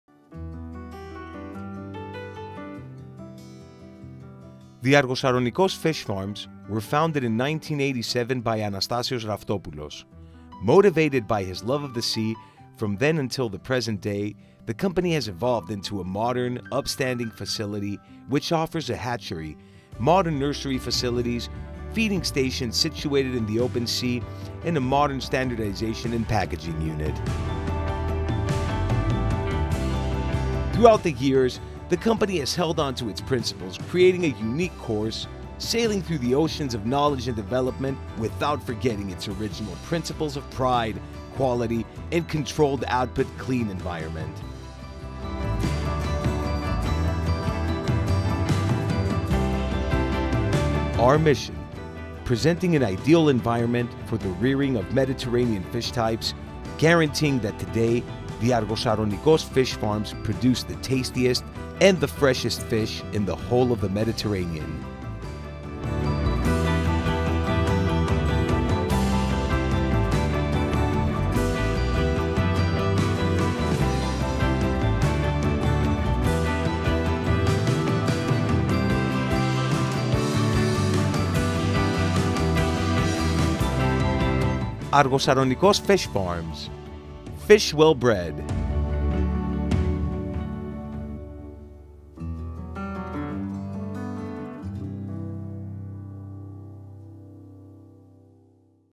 ARGOSARONIKOS FISH FARM
The Argosaronikos fish farms are active since 1987.
Focusing on the production of Mediterranean fish types sea bream, sea bass, and meagre. All our produce is exported to European countries and to USA.